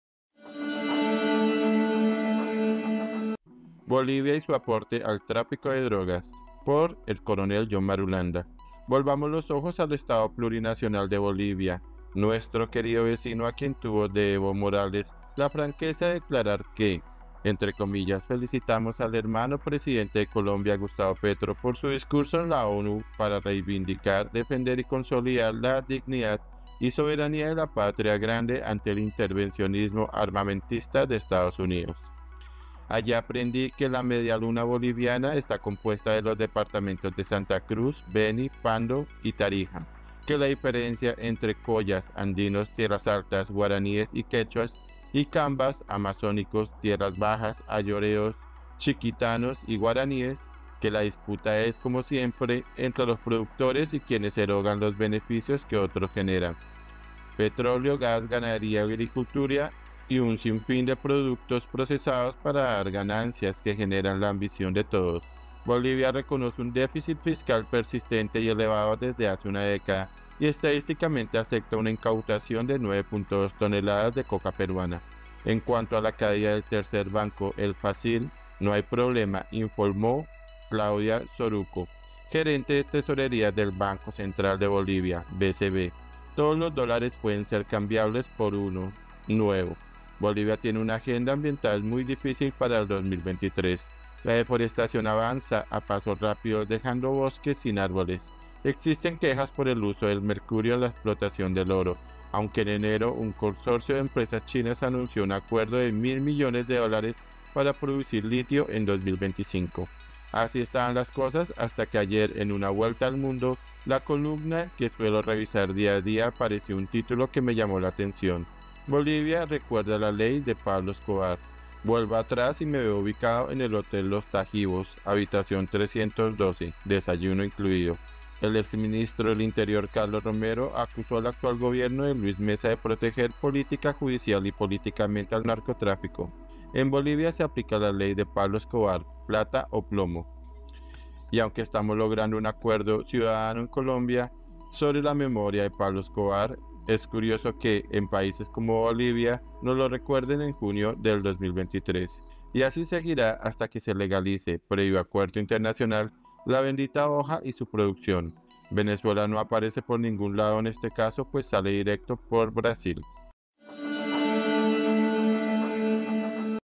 BOLIVIA Y SU APORTE AL TRÁFICO DE DROGAS 🔴 Por John Marulanda
Volvamos los ojos al Estado plurinacional de Bolivia (Mamallacta, Marka y Mborivia) , nuestro “querido” vecino a quien tuvo de Evo Morales, la franqueza de declarar que “Felicitamos al hermano presidente de Colombia, Gustavo Petro, por su discurso en la ONU para reivindicar, defender y consolidar la dignidad y soberanía de la patria grande ante el intervencionismo armamentista de Estados Unidos (…)”.
columna John Marulanda